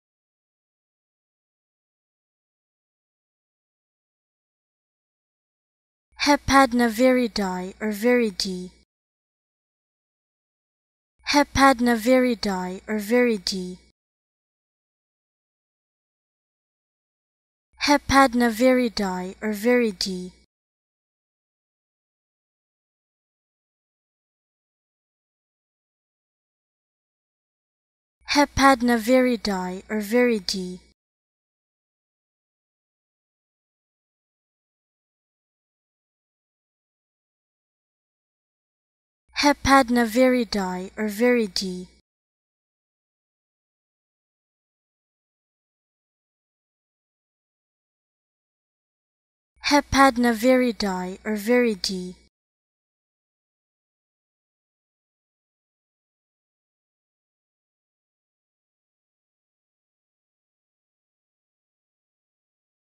How To Say Hepadnaviridae
Learn how to say Hepadnaviridae with EmmaSaying free pronunciation tutorials.